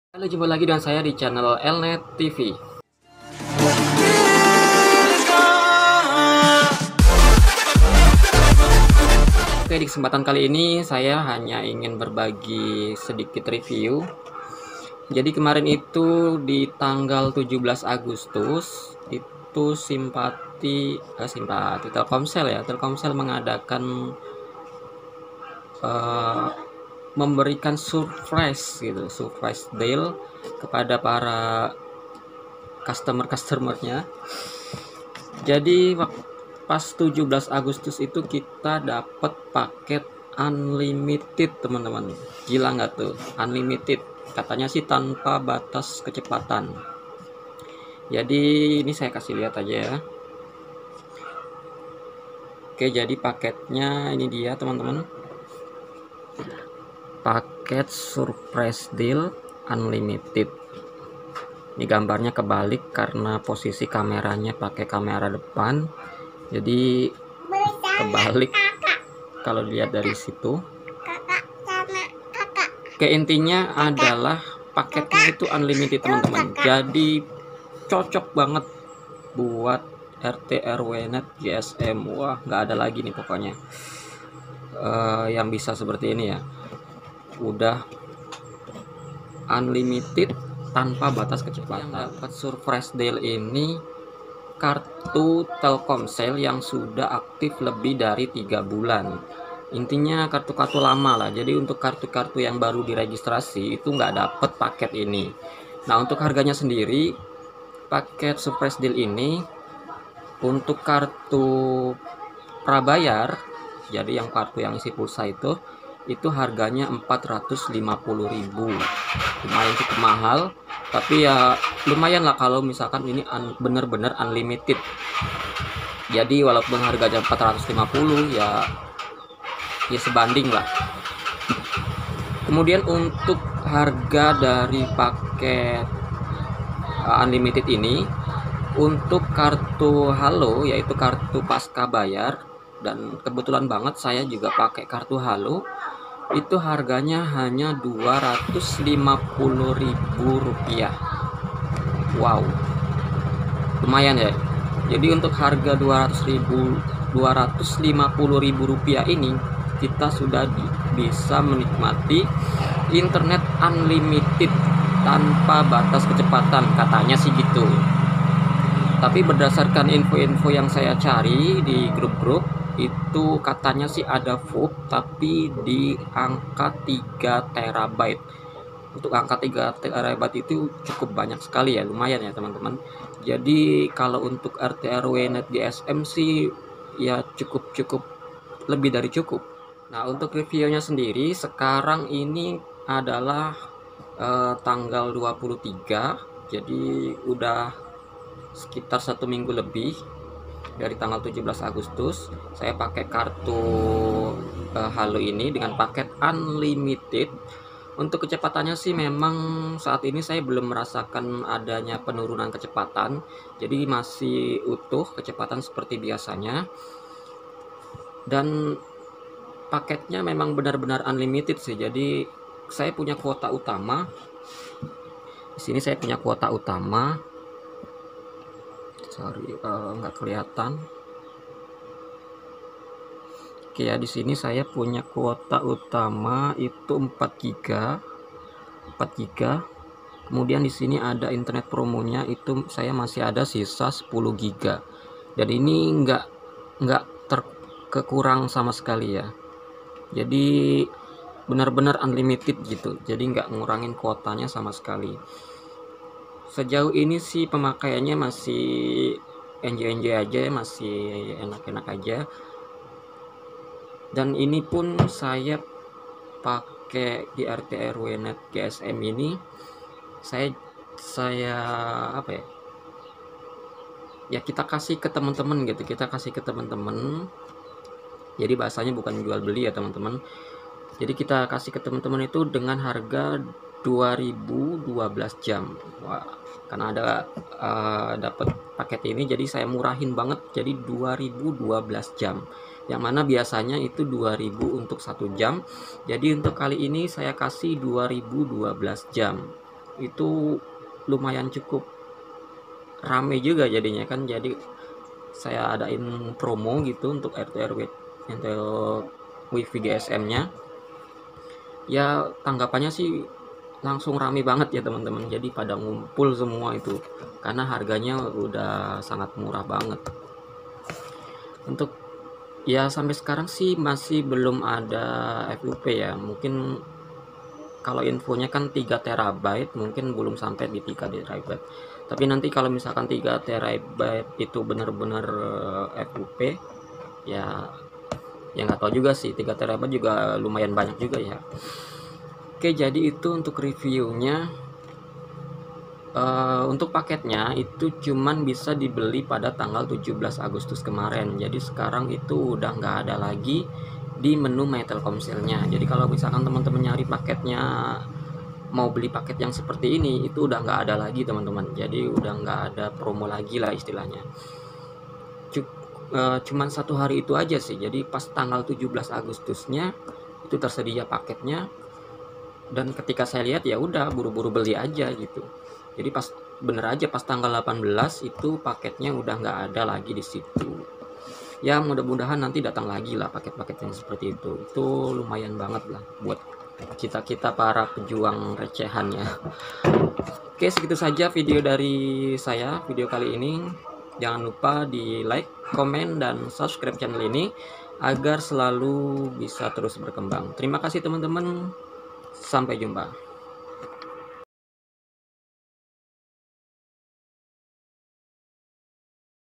riview paket unlimited telkomsel suprise deal agustus
rt rw net dengan internet dari GSM adalah sebuah gugusan ide masyarakat dalam mewujudkan internet menjadi murah (non comercial) dengan sistem iyuran untuk pembelian paket kuota internetnya.